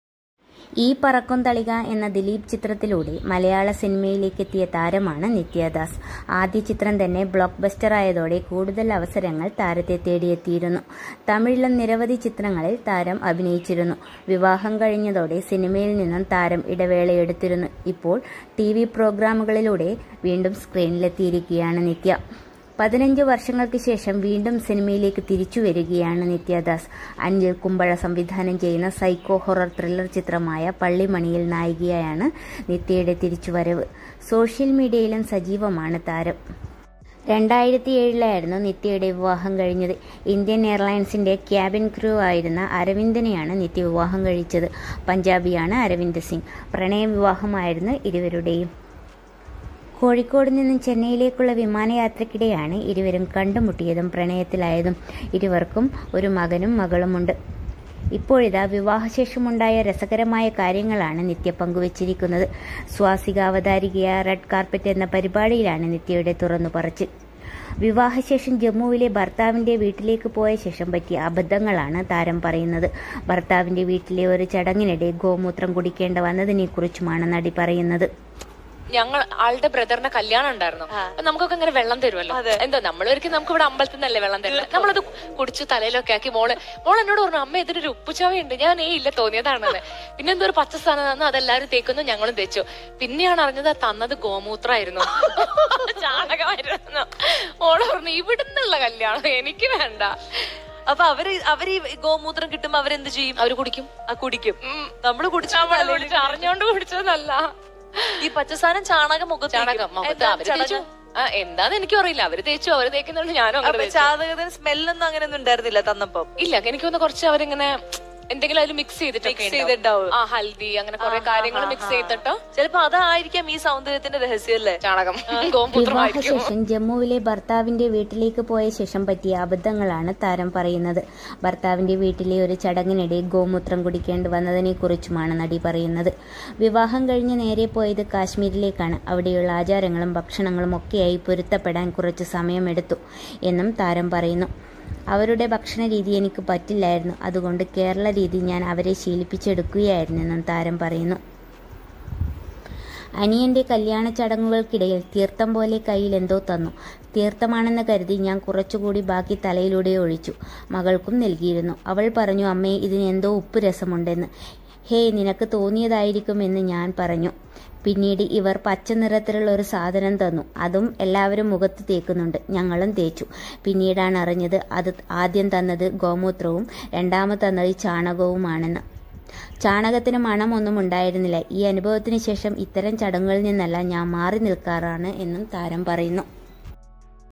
ഭർത്താവിന്റെ വീട്ടിൽ വെച്ച് തീർത്ഥം പോലെ എന്തോ കയ്യിൽ തന്നു…തീർത്ഥം ആണ് എന്ന് കരുതി ഞാൻകുടിച്ചു...
ഗോമൂത്രം കുടിച്ചു, ചാണകം മുഖത്ത് തേച്ചു!!! ഭര്‍തൃവീട്ടിലെ ആചാരങ്ങള്‍ തുറന്ന് പറഞ്ഞ് നിത്യാ ദാസ്